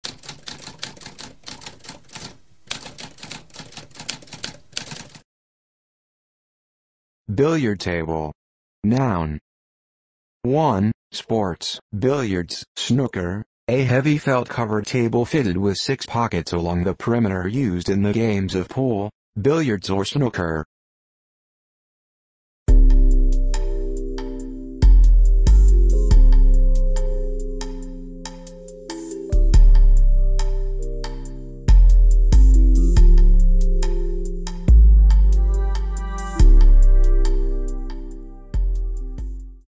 What does billiard table mean?
A spoken definition of billiard table.

Intro Sound:
Typewriter - Tamskp